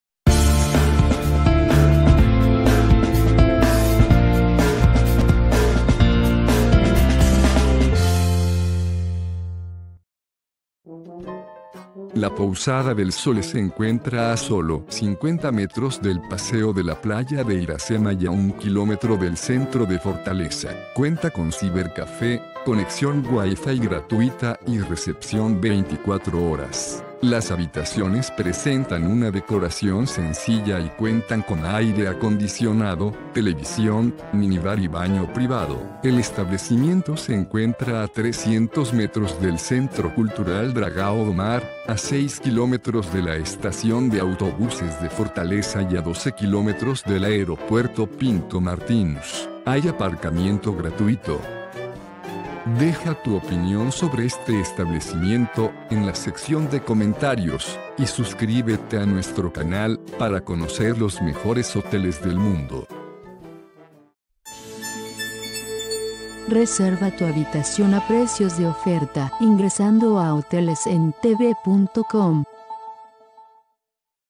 Hostal Pousada Del Sole, Fortaleza
Pousada Del Sole incluye recepción 24 horas, habitaciones no fumadores y servicio de lavandería. El edificio de Pousada Del Sole se encuentra ubicado en Av. Almirante Barroso, 966 y el equipamiento de sus 9 habitaciones se compone de minibar, ducha y tv.